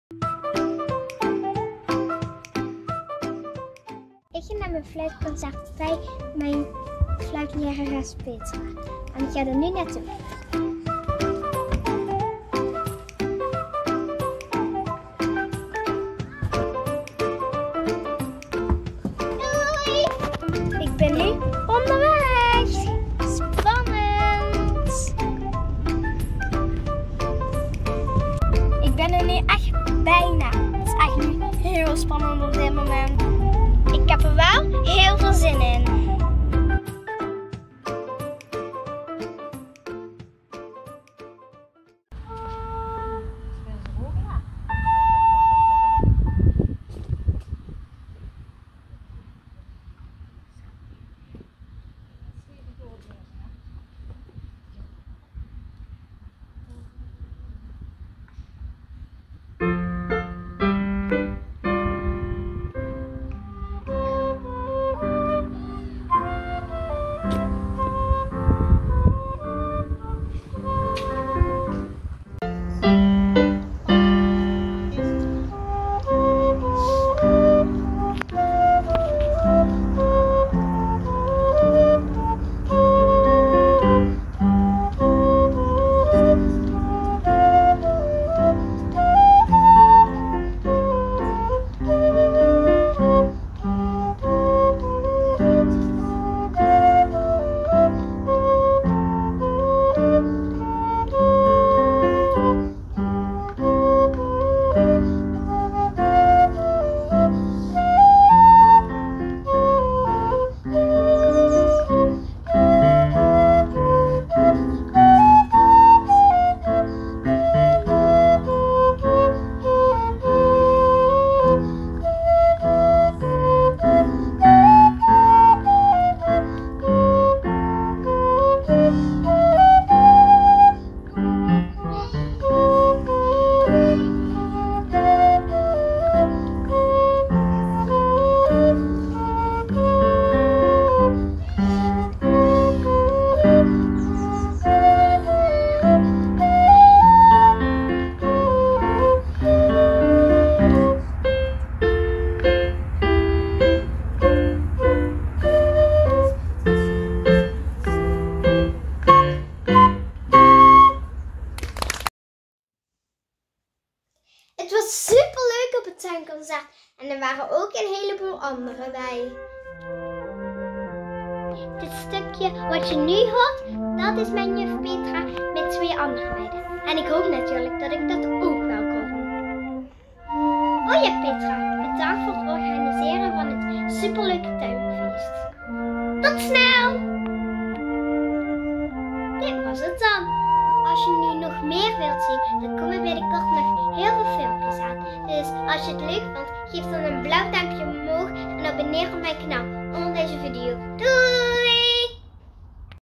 Mijn dwarsfluit 15 Tuinconcert
10 juli 2021 - Tuinconcert bij mijn dwarsfluitlerares.
Heel erg leuk om te doen!